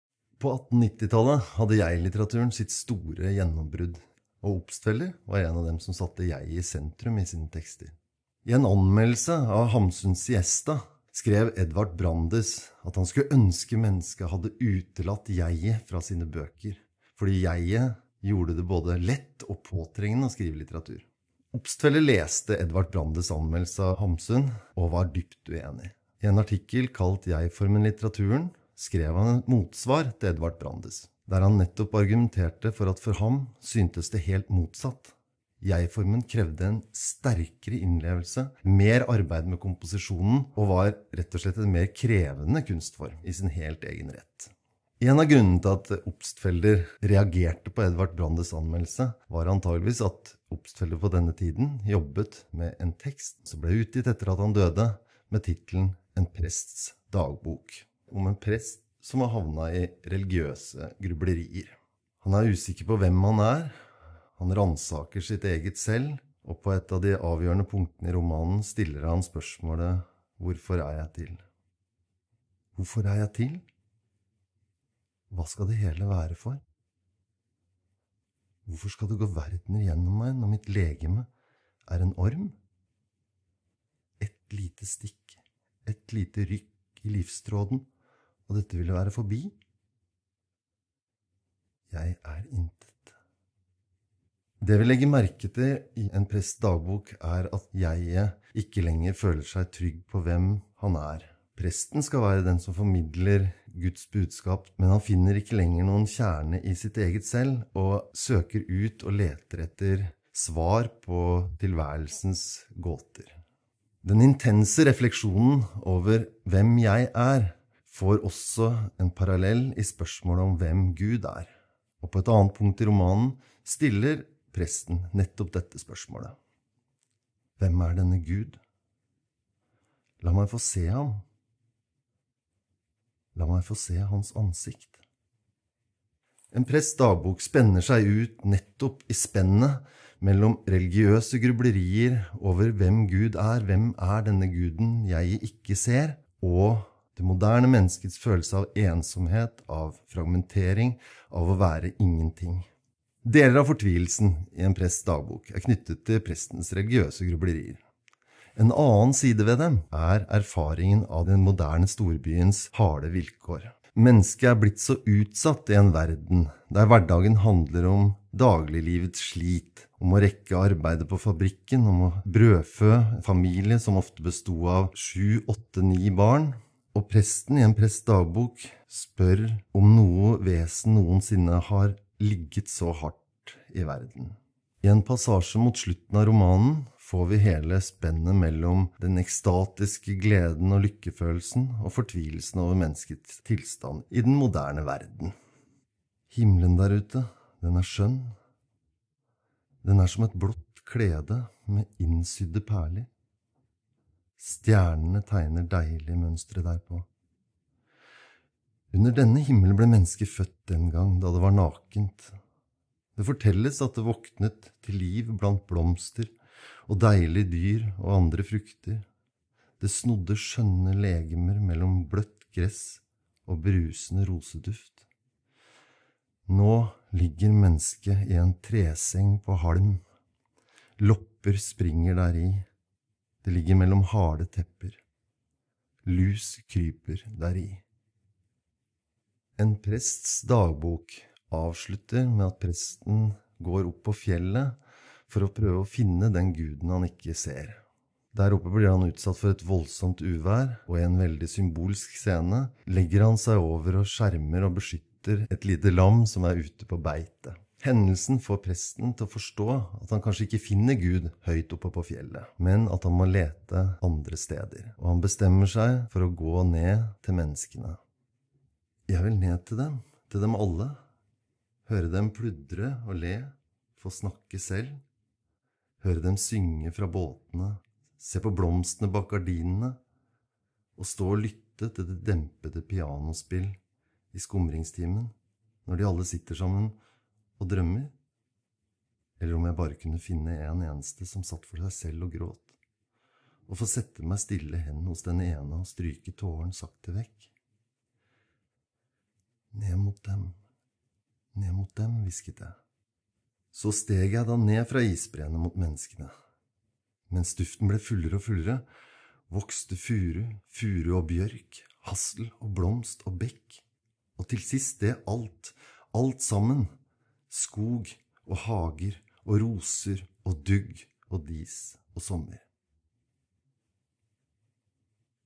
En Prests Dagbok av Sigbjørn Obstfelder. Fortalt av Trond Haugen.
I denne videoen får du et innblikk i «En Prests Dagbok» og kommer helt tett på originalmanuskriptet til romanen.

Sigbjørn Obstfelders forfatterskap markerer modernismens inntog i norsk litteratur. For ham var «mennesket blitt en ny skapning», og i sin diktning skildrer han det på stadig nye måter.

«Verden er ny for mig», skrev Obstfelder. Det er også grunnen til at han har betydd mye for senere forfattere. Hans opplevelse av å være et porøst jeg i en verden i forandring gjør ham til en av våre samtidige.

Utstilling i Nasjonalbiblioteket 30. september – 3. desember 2016. Utstillingen «Sansen for det moderne» setter hans tekster om ensomhet, reiser og storbyer inn i en større historisk sammenheng. Ved hjelp av et rikt utvalg brev, skrivehefter, aviser, bøker og fotografier forteller utstillingen Obstfelders biografi fra oppvekst i Stavanger til hans tidlige død i København. I sentrum står hans jakt på å gi det det myldrende moderne liv en litterær form.